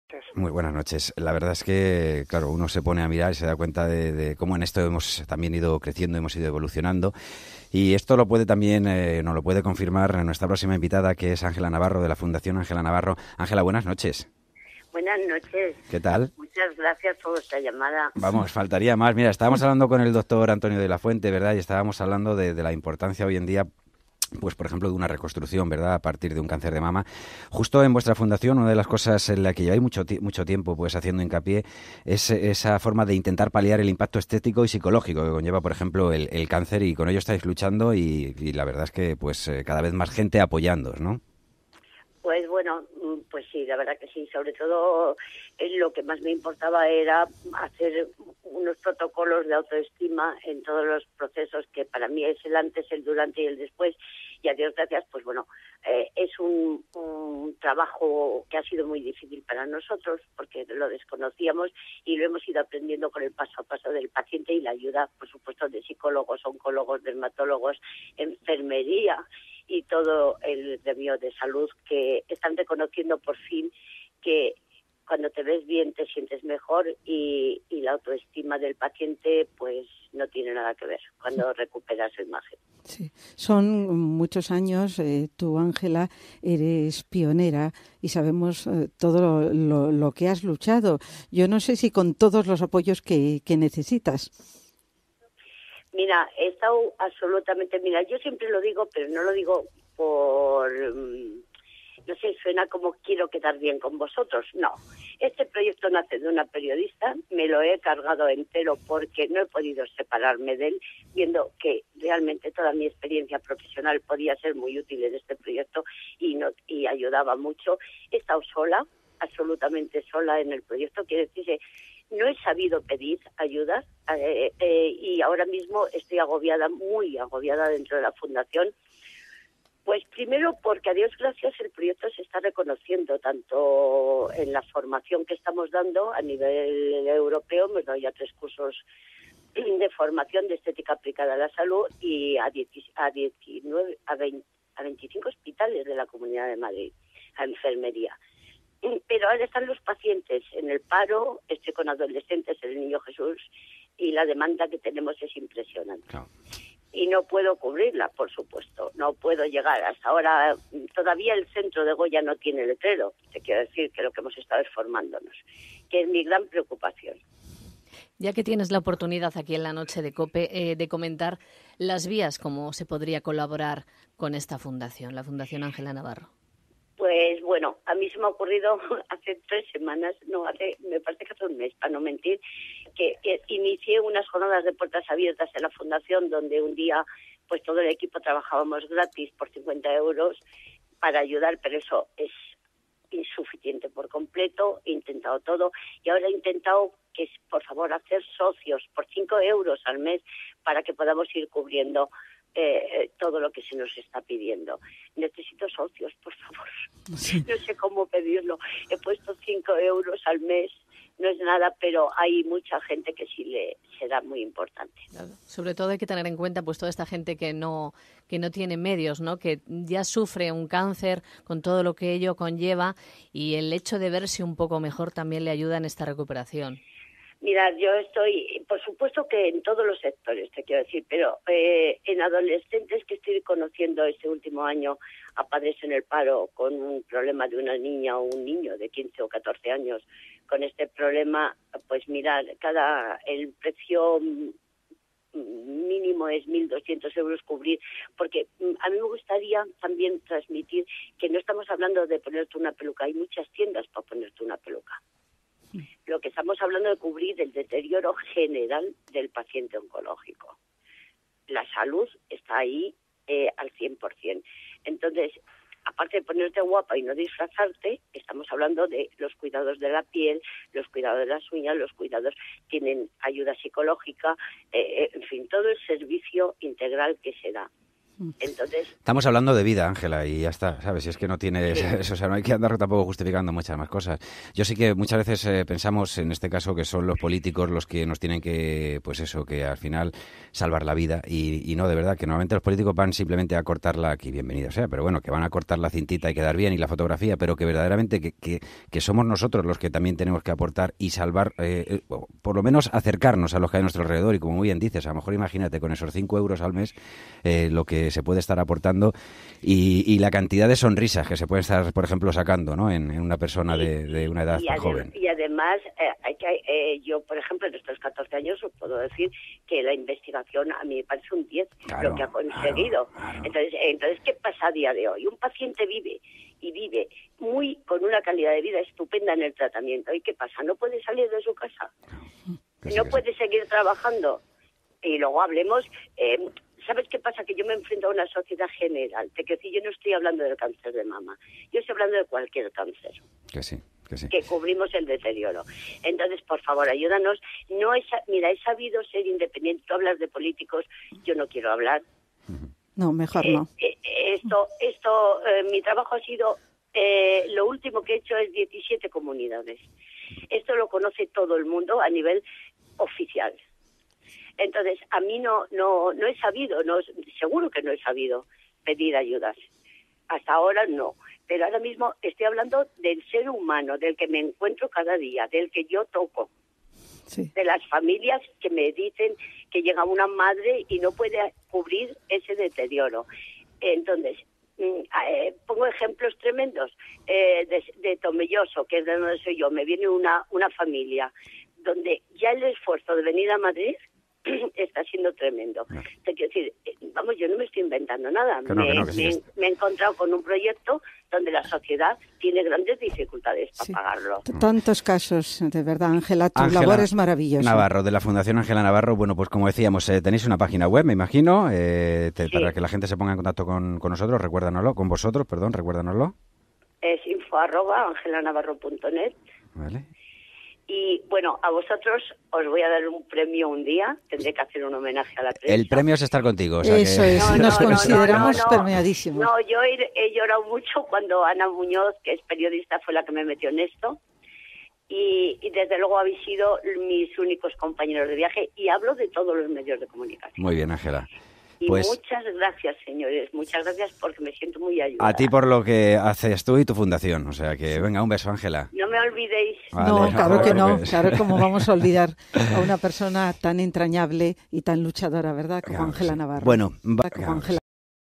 Angela Navarro en Cadena Cope
Clipping con la entrevista de Angela Navarro en la Cadena Cope con motivo del Día Mundial contra el Cáncer de Mama.